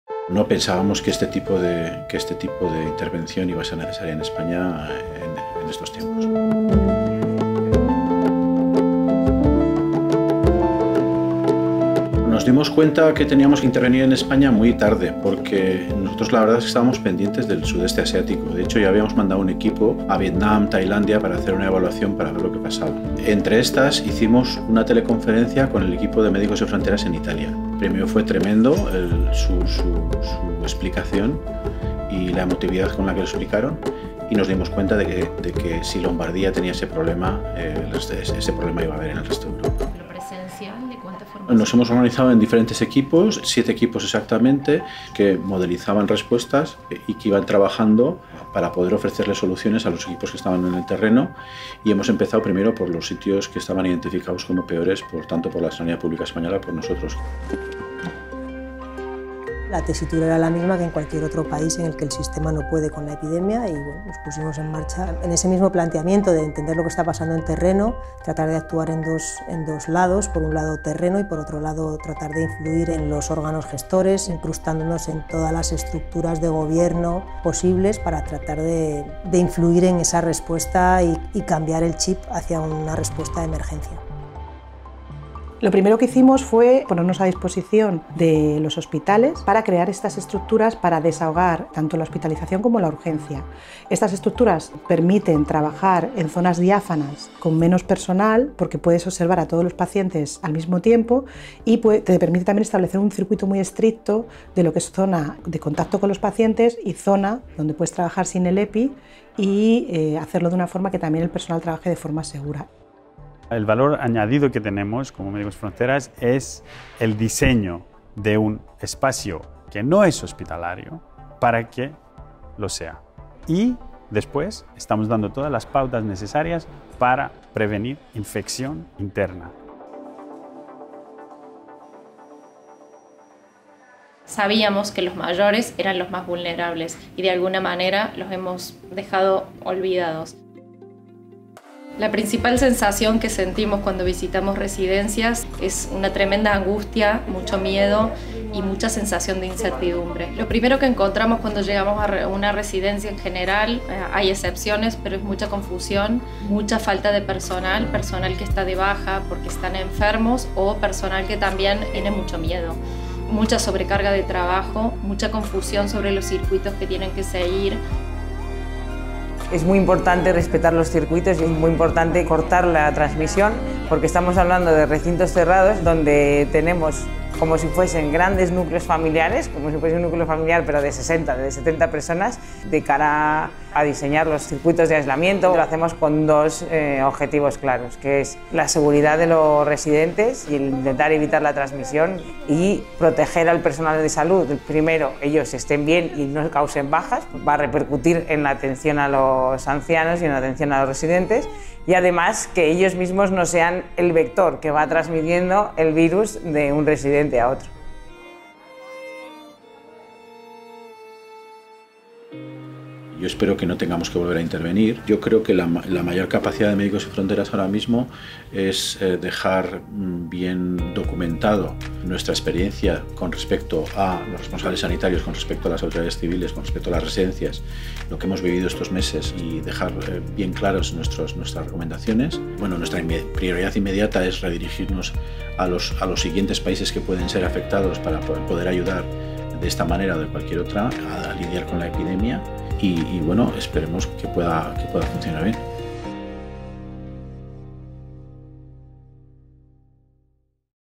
COVID-19: así ha sido nuestra intervención en España
Una intervención que, gracias a tu colaboración, ha podido ponerse en marcha y prestar ayuda, de persona a persona, esta vez aquí, en casa, cuando ha sido más necesario.